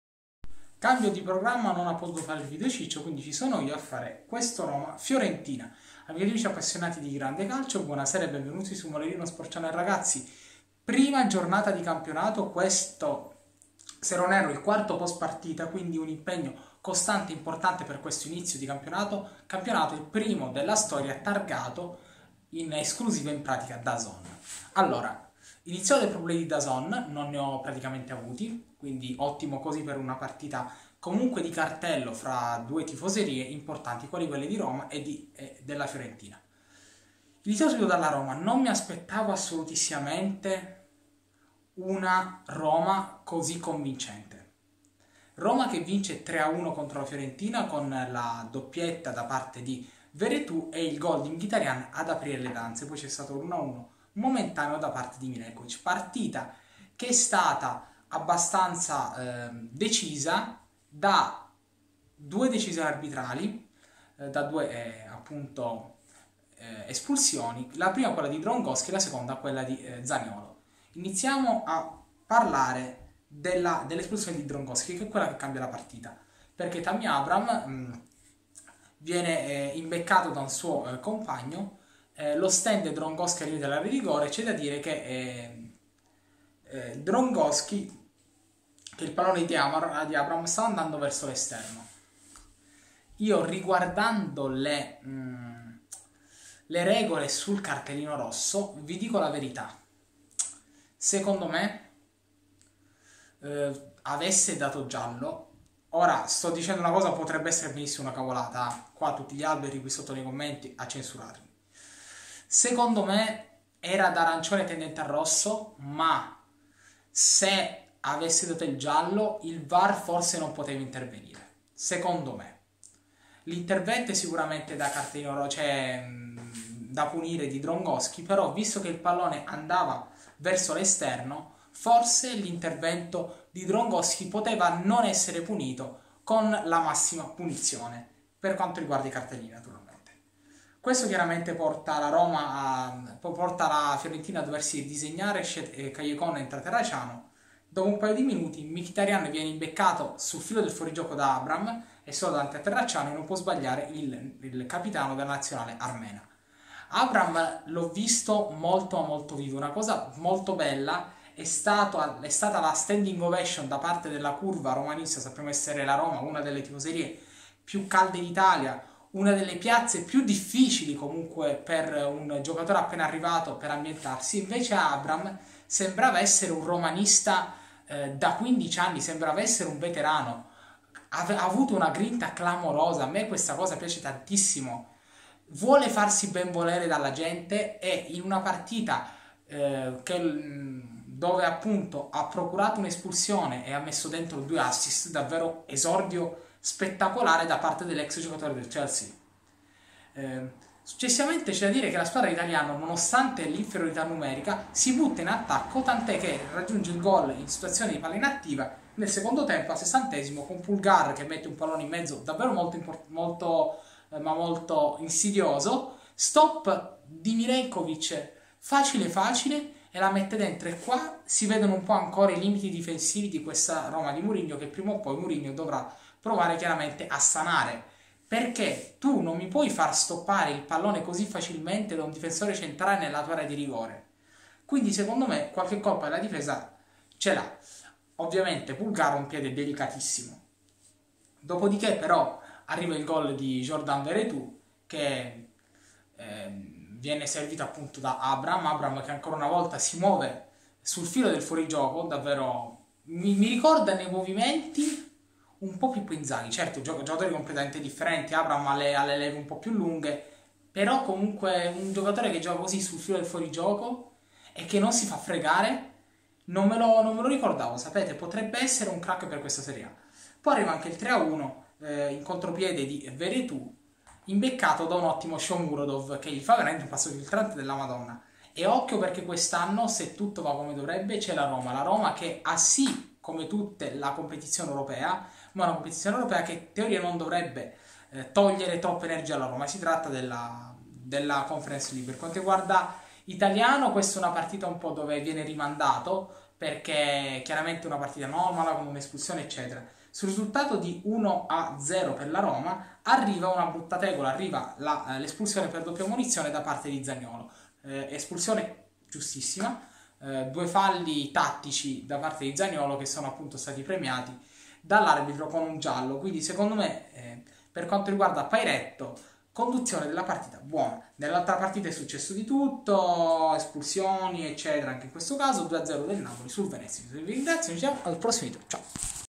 ROMA FIORENTINA 3-1: UN SONTUOSO VERETOUT LA DECIDE. SERIE A 2021/2022
Gol Roma Fiorentina
  Roma Fiorentina